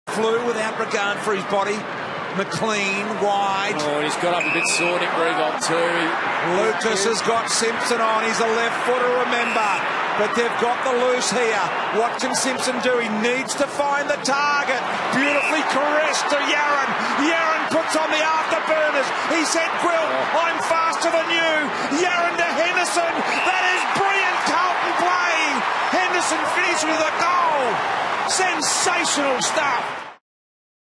Blues go coast to coast - AFL
Chris Yarran burns up the Etihad turf to supply Lachie Henderson with an easy goal.